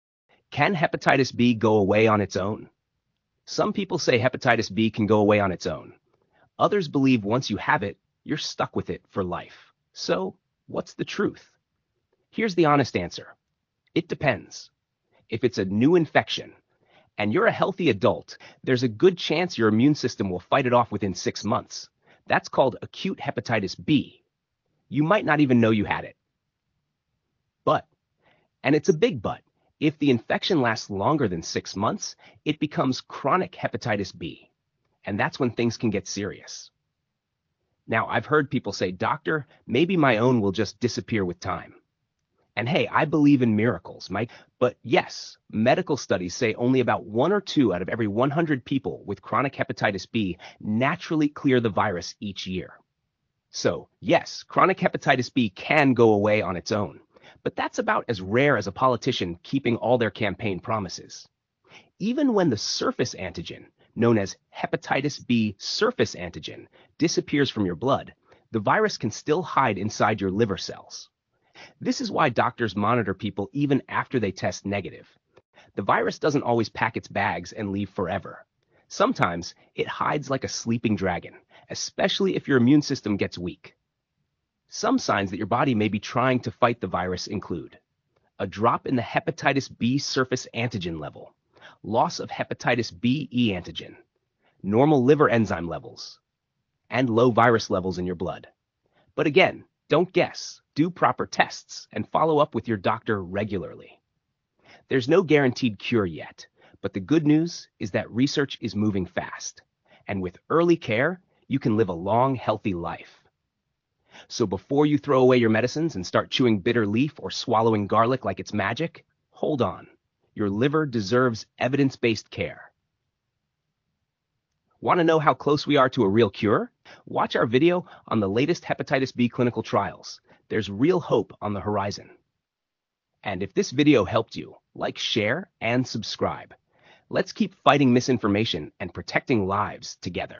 Can Chronic Hepatitis B Go Away on Its Own? Dr Kayode Explains the Rare Truth
Many people believe that chronic Hepatitis B is a lifelong sentence — but is that always true? In this video, Dr. Kayode explains whether chronic Hepatitis B can go away on its own, how rare spontaneous clearance is, and why regular follow-up is vital even if you feel healthy.

🧪 Learn what HBsAg clearance means
📉 Discover the real chance of spontaneous healing
🩺 Understand what a "functional cure" is and what it isn’t

If you’re concerned about your symptoms, consult a healthcare provider today. you can also get a quick, reliable test done by following this IF you are in the US.

PubMed Here are credible references from trusted medical sources and peer-reviewed journals that support the information about the possibility of chronic Hepatitis B clearing on its own
World Health Organization (WHO):
Overview of Hepatitis B infection, acute vs. chronic, and treatment options.

Answers common questions about Hepatitis B, including spontaneous clearance.

Detailed explanation of the five phases of chronic Hepatitis B and how spontaneous HBsAg loss occurs.

European Association for the Study of the Liver (EASL):
Clinical guidelines showing the annual rate of spontaneous HBsAg loss is between 0.5% to 2%.

Study showing the natural annual clearance rate of Hepatitis B surface antigen in chronic HBV carriers.

In-depth medical review of how chronic Hepatitis B progresses and how spontaneous regression can occur in rare cases.